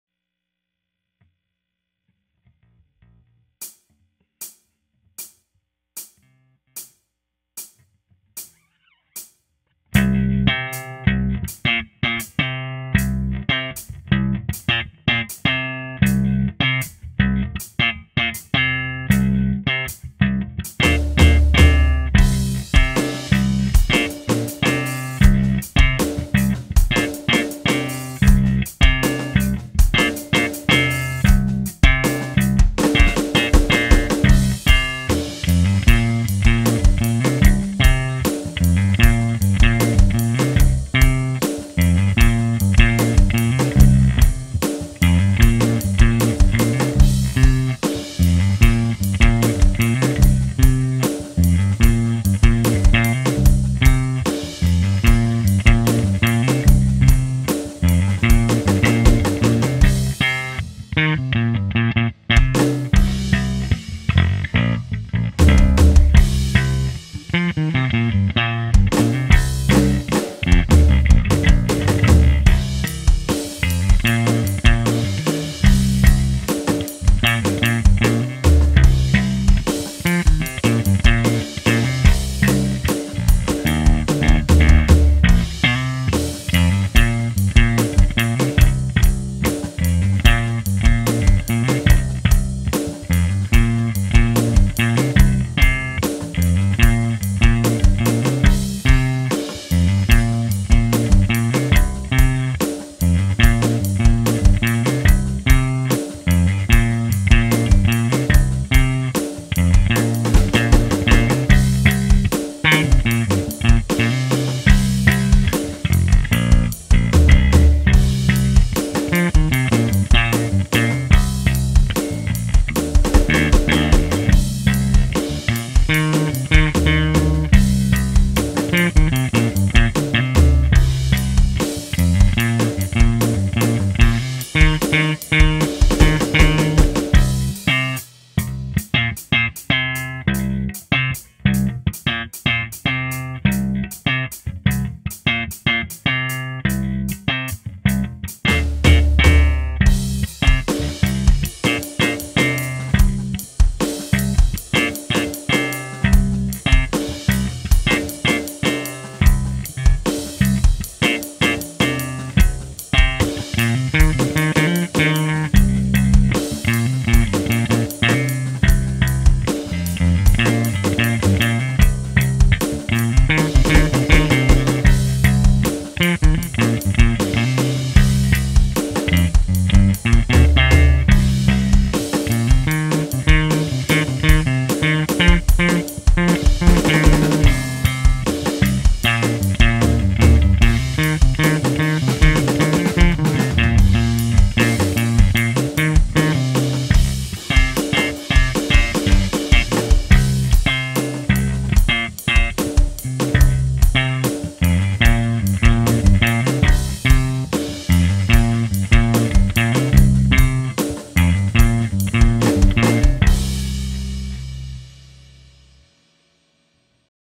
Starting to write songs for next album with the Bass.
Focusing more on the bass for album 2. Here is a demo of a song we are working on. We will be adding guitars and eventually vocals. Just sharing the process as we go.